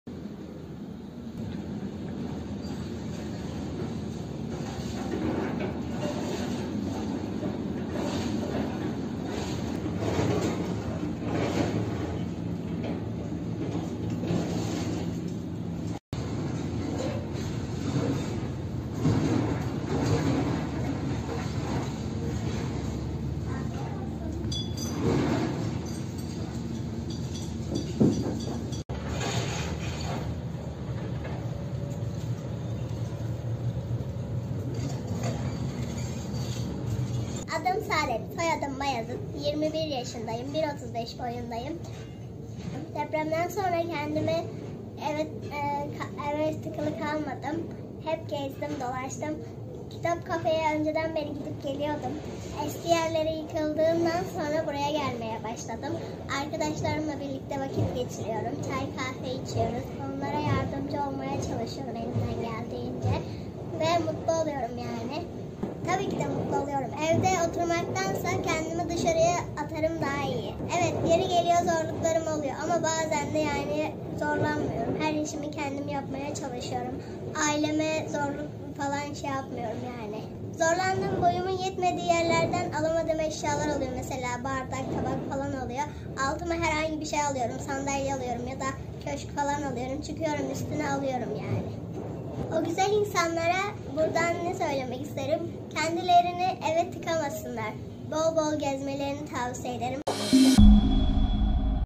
1 metre 35 cm boyundaki Saadet, her şeye rağmen zorlukları aşıyor
Kahramanmaraş'ta yaşayan 1 metre 35 cm boyundaki 21 yaşındaki Saadet Bayazıt, yaşadığı zorluklara rağmen sosyal hayatında aktif olarak insanlara örnek oluyor.
Laron Tipli Gelişme Geriliği hastalığıyla mücadele eden 21 yaşındaki Saadet, yaşadığı zorluklara rağmen çalışarak hayata tutunmaya çalışıyor. Kahramanmaraş’ta deprem sonrasında da evde kapalı kalmayıp birçok etkinliklere katıldığını söyleyen Bayazıt, katılım sağladığı kitap kafede, çay servisi yapıyor, masayı siliyor, müşterileri karşılıyor.
Boyundan dolayı hayatında yaşadığı zorlukları kendi başıma aşmaya çalıştığını söyleyen Bayazıt ,"Bu konuda aileme dahi zorluk çıkarmıyorum" dedi.
Sosyal yaşamda aktif oldukça çok mutlu olduğunu söyleyen Bayazıt, "Ben 1 metre 35 cm boyundayım. Depremden sonra da evde kapalı kalmadım. O süreçte de hep dolaştım. Kitap kafeye geliyorum. Eski yerleri depremde yıkıldı ama ben şuan buraya geliyorum. Burada arkadaşlarımla birlikte vakit geçiriyorum. Burada çay kahve içiyoruz. Elimden geldiğince onlara yardımcı oluyorum. Bir şeyler yaptıkça sosyal oldukça çok mutlu oluyorum" diye konuştu.
Bayazıt, boyumdan dolayı zorlandığı zamanlar olduğunu dile getirerek, “Ama her işimi kendim yapmaya çalışıyorum. Bu konuda aileme zorluk çıkarmıyorum. Kendi işimi kendim halletmeye çalışıyorum. Çok zorlandığım konu ise eşyalar konusunda oluyor. Bardak, tabak falan almak istiyorum ama boyum yetişmediği için alamıyorum. Tabi o sorunu da sandalye alarak çözmeye çalışıyorum. Benim gibi olan insanlara buradan tavsiyem, kendilerini eve kapatmasınlar bol bol gezmelerini tavsiye ediyorum" dedi.